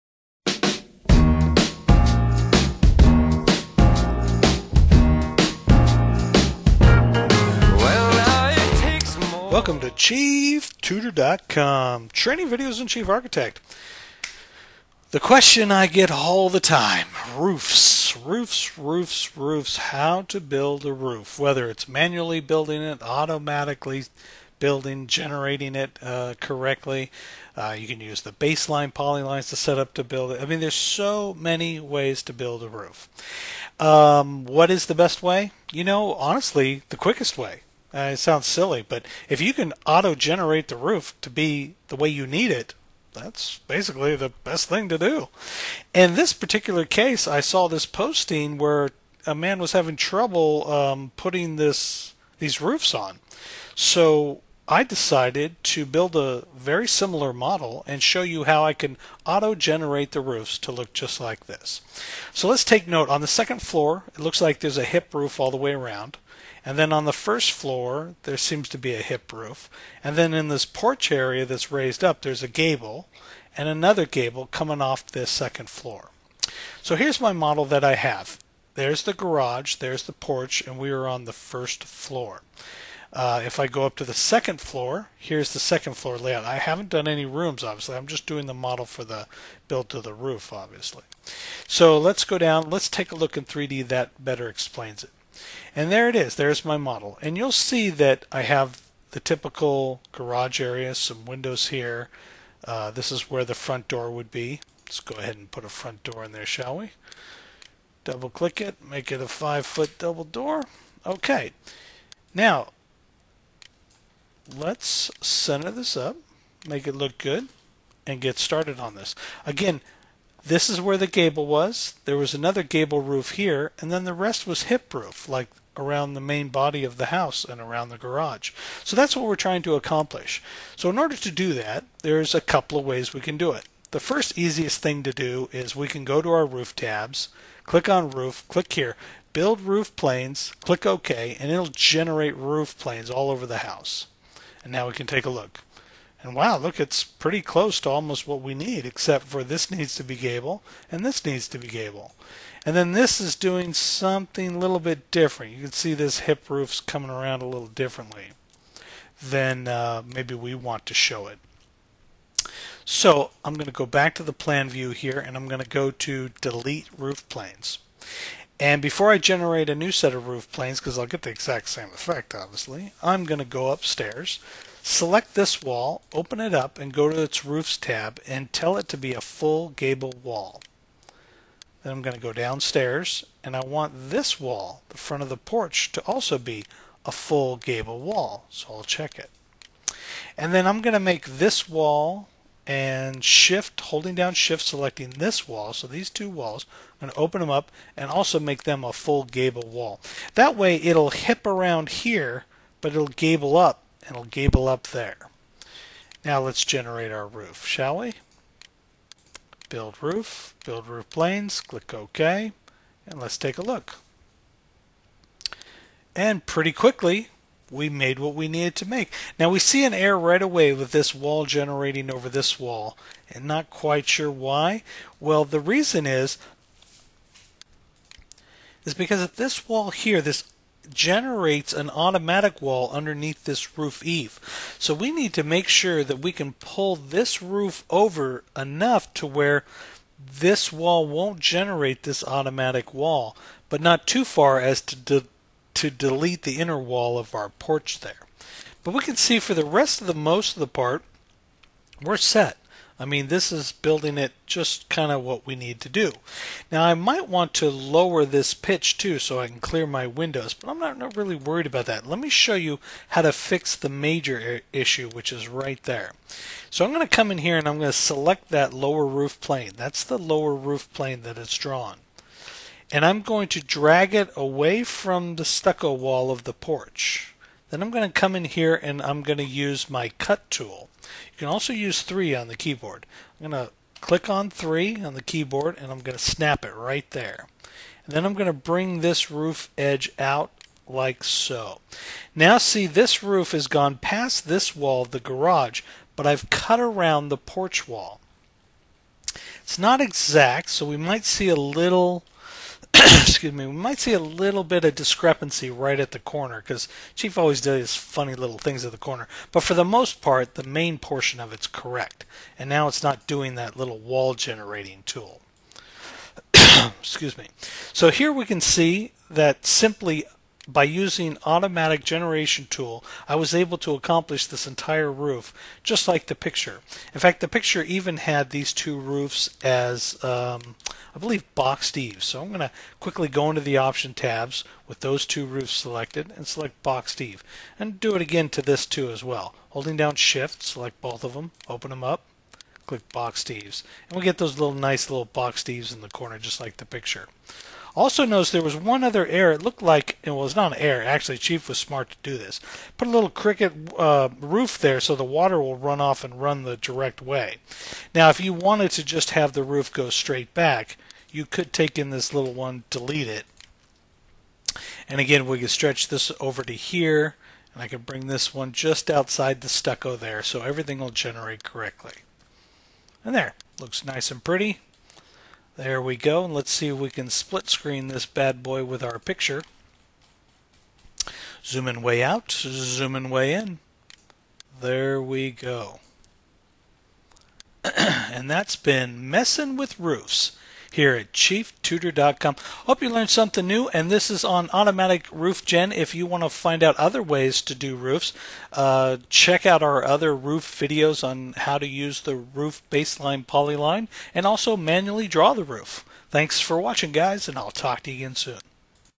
Automatic Roof Generating Tool in Chief Architect X3
This is a quick down and dirty HowTo Video that will take up most of the day.